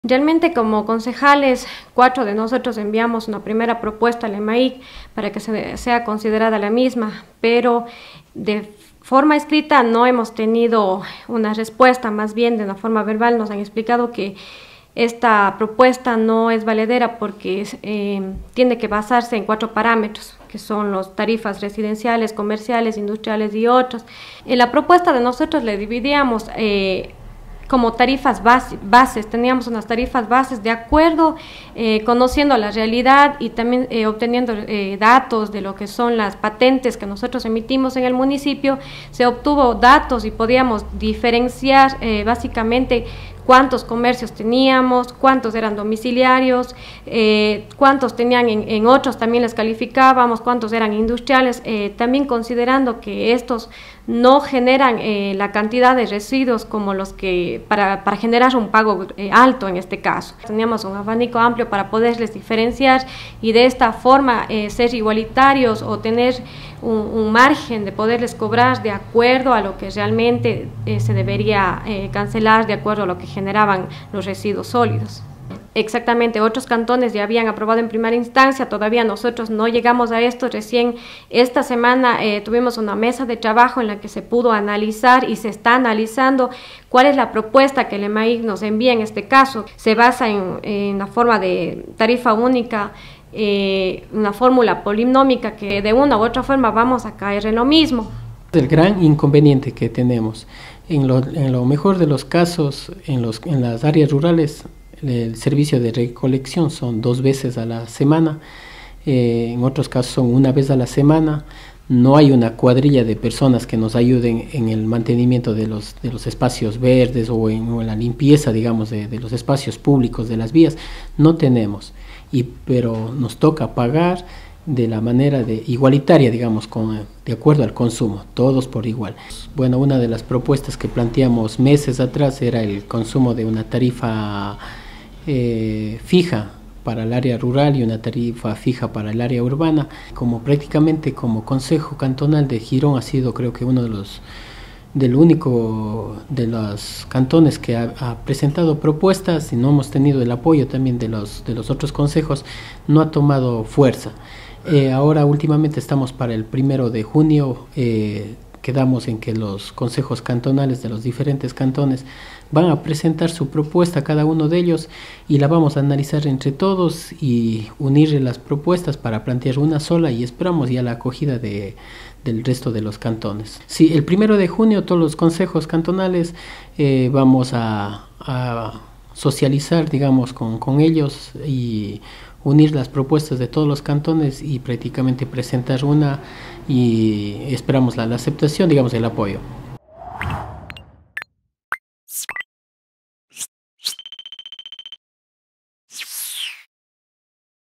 Parroquias pagan igual que los cantones por el servicio de recolección pero por servicio disminuido
Los dos concejales rurales del cantón Girón, argumentan que para sus sectores el problema del servicio de recolección es mayor puesto que pagan igual que en el área urbana, pero recibiendo el servicio en el mejor de los casos en un 40 %. Mary Brito y Mauricio Cajamarca proponen tarifas fijas para los cantones y parroquias.